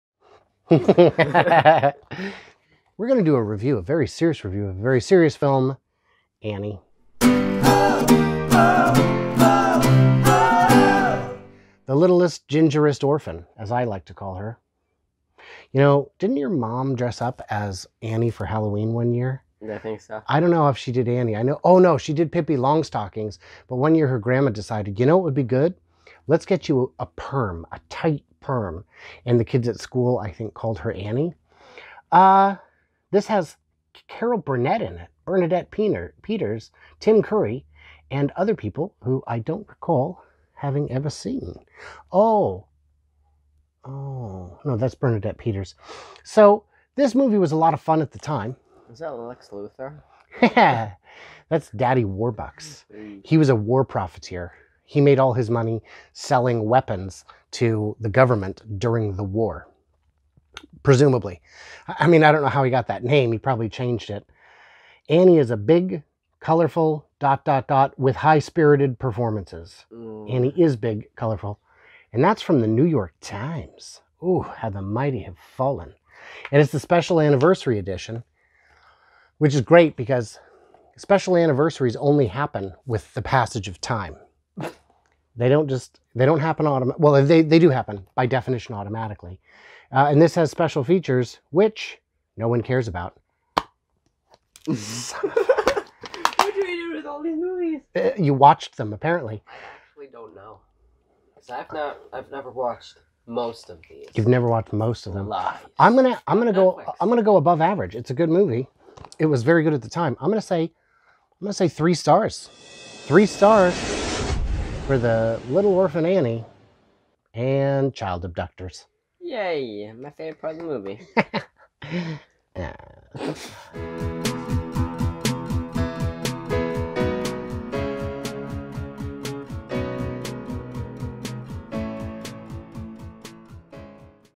Annie the Musical (1982) Film Review
Annie is a 1982 American musical comedy-drama film based on the Broadway musical of the same name by Charles Strouse, Martin Charnin and Thomas Meehan, which in turn is based on the Little Orphan Annie comic strip created by Harold Gray. Directed by John Huston and written by Carol Sobieski, the film stars Albert Finney, Carol Burnett, Ann Reinking, Tim Curry, Bernadette Peters, Geoffrey Holder, Edward Herrmann and Aileen Quinn as the title character.

If you think you're too cool to watch a musical, rest assured you are just cool enough. Watch it ironically if you must, but dig in and enjoy because musicals are magical. I mean, they aren't rocket launches or baseball, but they can still stir some emotion.

If you want to collaborate or get reviewed, get in touch.